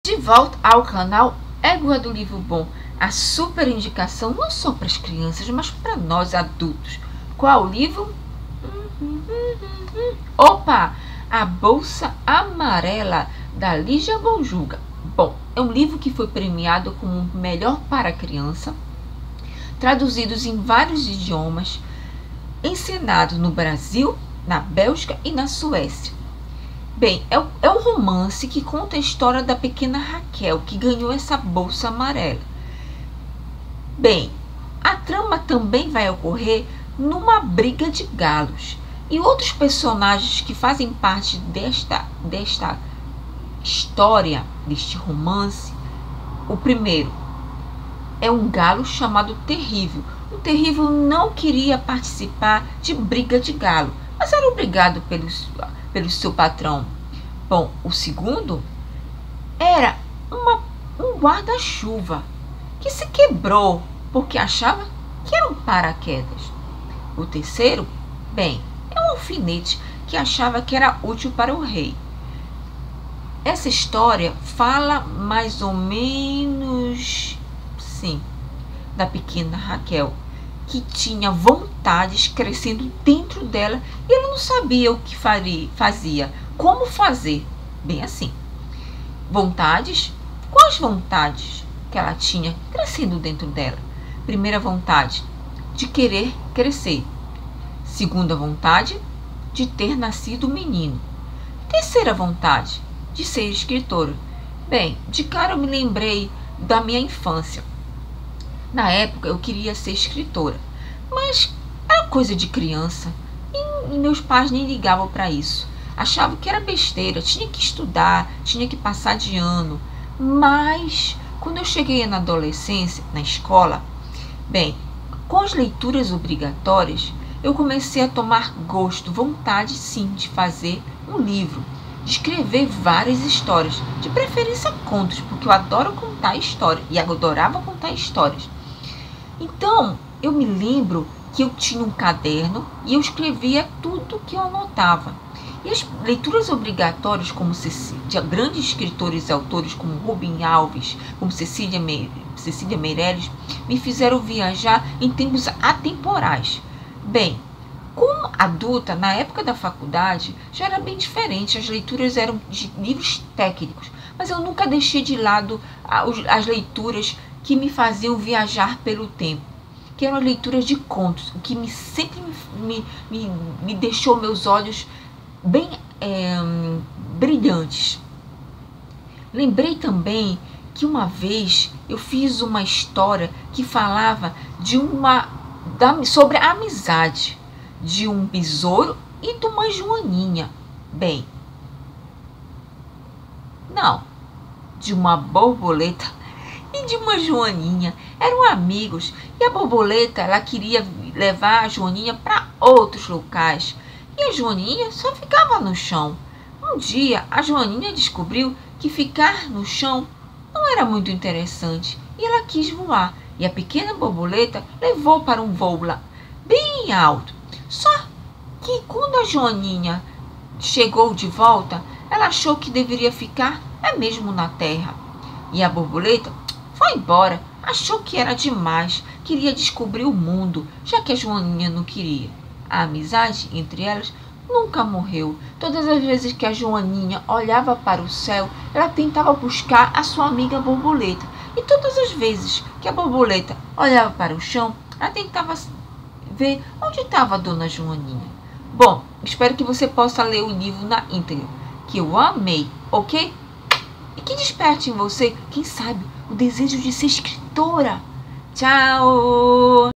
Ser escritora - Égua do Livro Bom!
Quais são as suas vontades? O que você deseja conquistar? Você deseja ser escritora? O livro transborda vontades de uma menina que deseja ser escritora.

Livro: A bolsa amarela
Escritora: Lygia Bojunga
Editora: Casa Lygia Bojunga; 35ª edição

95► super dica de leitura!

Vale ser lido na integra!
Adaptação e edição de vídeo: Alda de Cássia